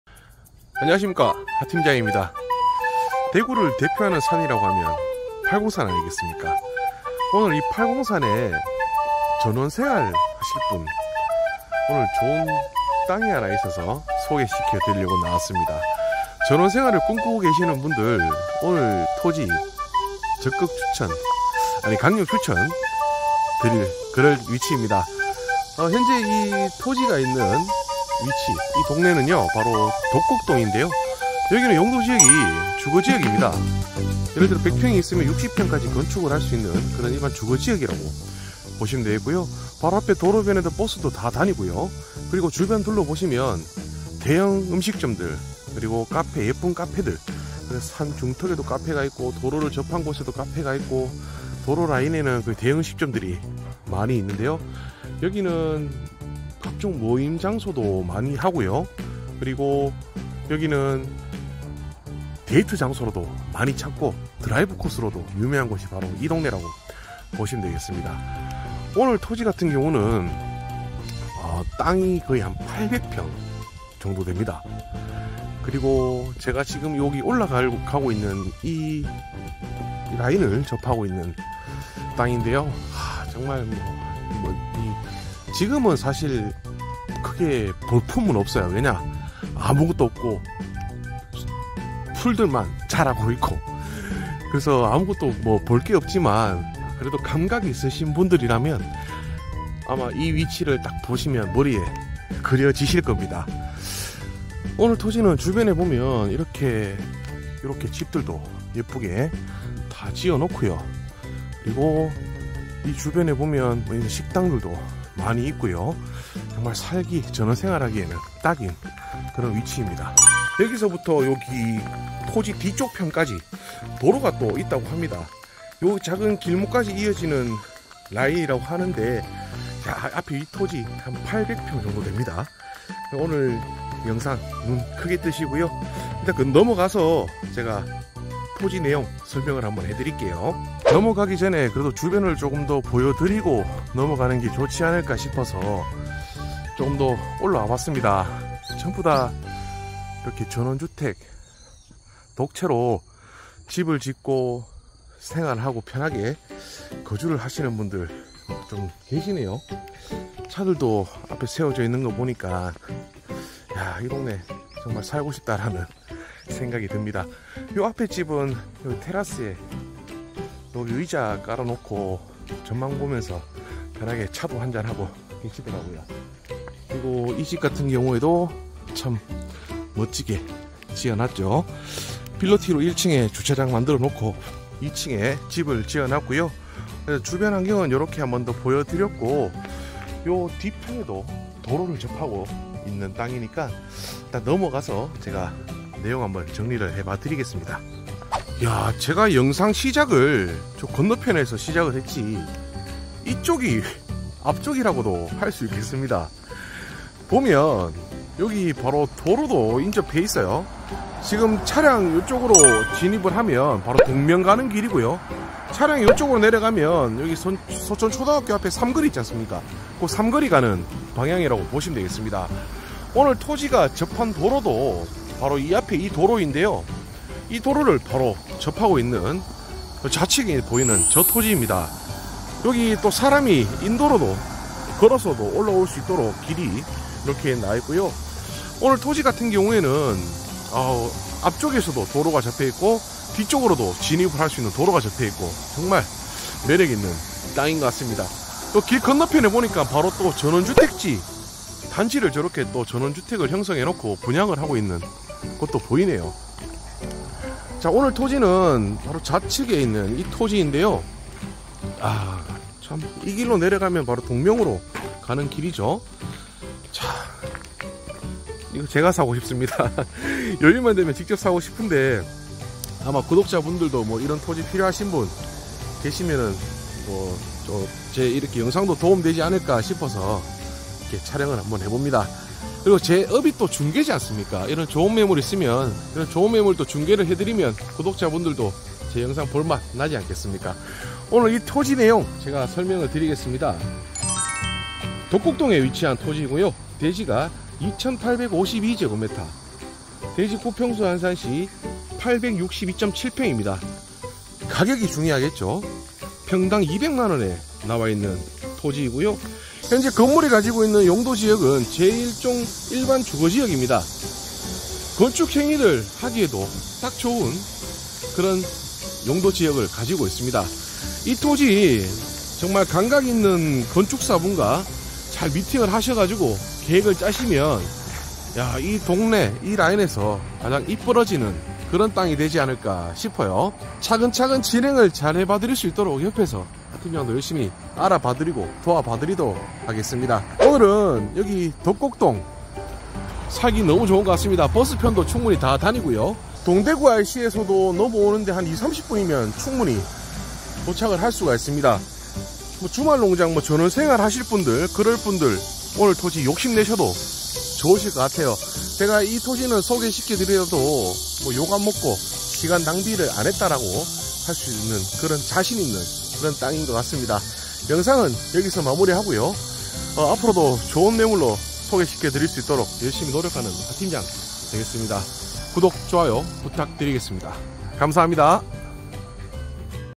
[하팀장]대구토지매매 팔공산 862평 전원주택지,대형카페 추천 위치랑활용성이 너무 좋은데요??
-팔공산 위치한 전원주택,대형카페 등 어떤 업종도 잘어울리는 위치에
진입도로 확보된 멋진 땅이네요!

"수익형부동산전문" 공인중개사 하준영 입니다.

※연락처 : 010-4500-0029
※카카오톡 ID : 45000029 (검색)
※해당영상 매물번호:240822

▶소재지 : 대구광역시 동구 덕곡동
▷면적 : 대지 2852㎡

▶거래형태 : 매매
▷거래예정금액 : 17억2000만원(평당 200만원)
▶중개대상물의 종류: 제1종일반주거 지목: 전,임

◆대표:沈民貞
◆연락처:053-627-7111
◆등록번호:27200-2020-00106